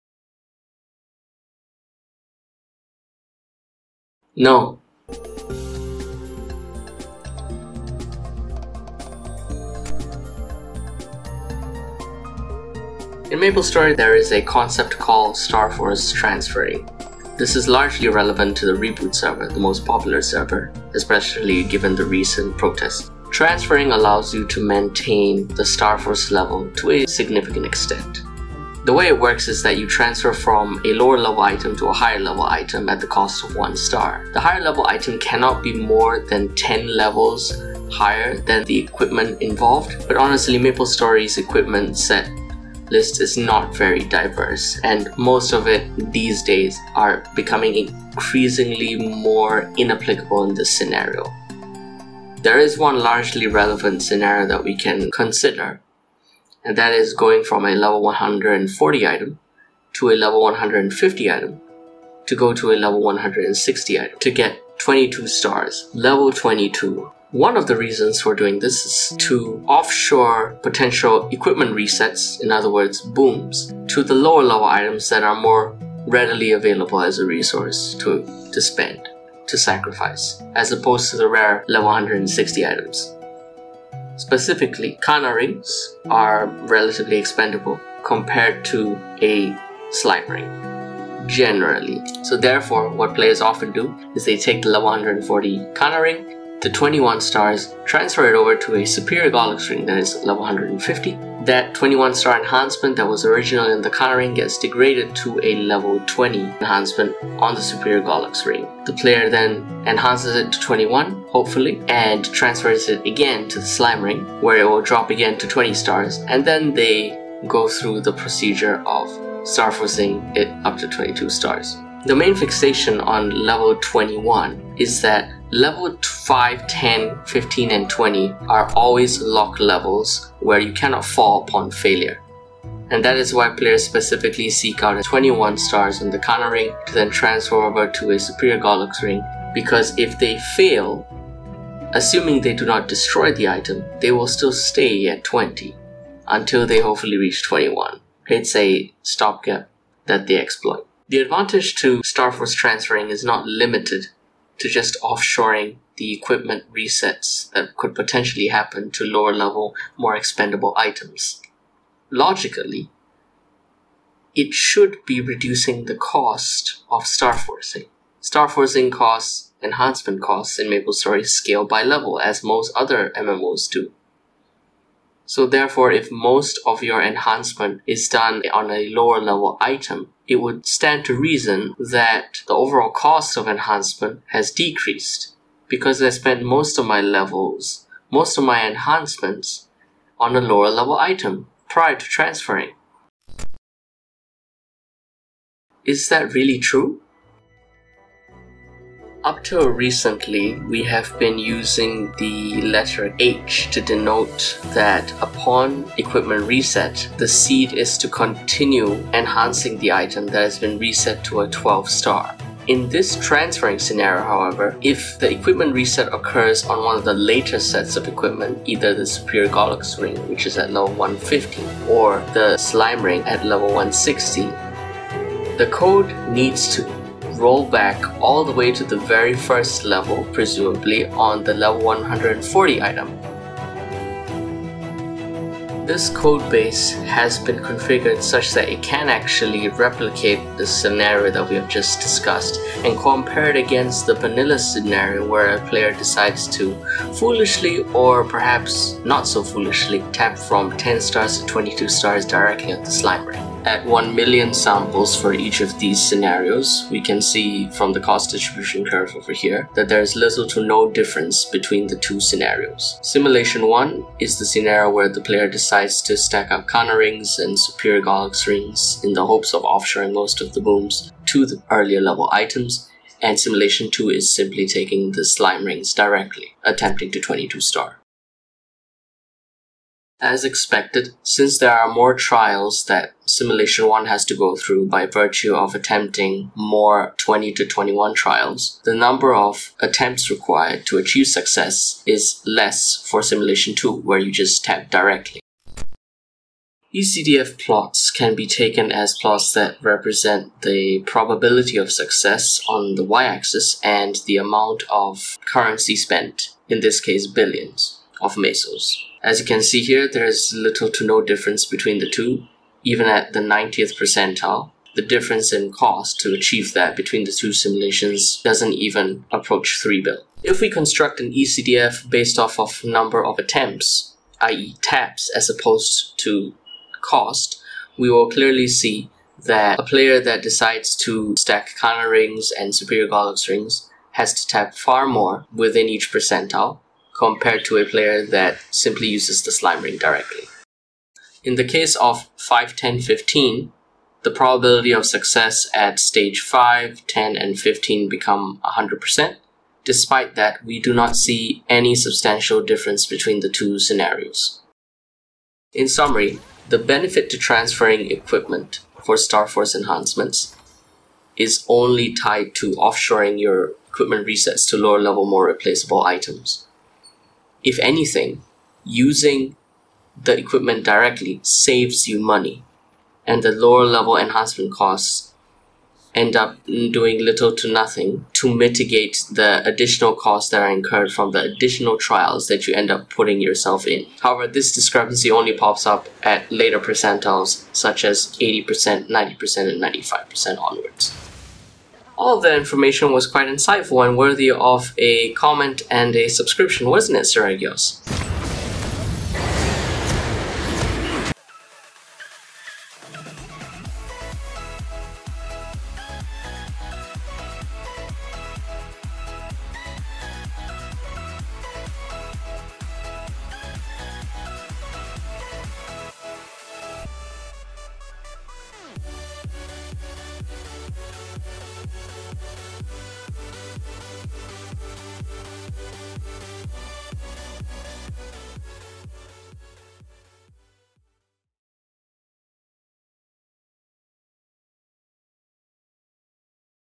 Boomstory | Does Transfer Hammer reduce starforcing cost? | Episode 4
The truth will SHOCK YOU. I swear. Results are for 1 million iterations for each scenario (which are compared directly with one another).

*Simulation 1*  Lvl 140 (10* to 21*) to Lvl 150 (20* to 21*) to Lvl 160 (20* to 22*)
*Simulation 2*  Lvl 160 (10* to 22*)

Characters wearing equips (except for Ethernel) were made from maples.im

Timestamps:
0:03 My Answer
0:14 Intro to transfer hammering
1:00 Prologue | Why people transfer hammer from lvl 140 to 150 to 160
2:32 Argument | Transfer hammering decreases cost of starforcing
3:15 Rebuttal | Challenging this argument
3:19 Building the Bernoulli trial inputs for the Advanced Trial Simulator Programme
3:56 Results | Off event comparison
5:41 Results | During 5/10/15
5:55 Summary
6:31 Comment and Subscribe!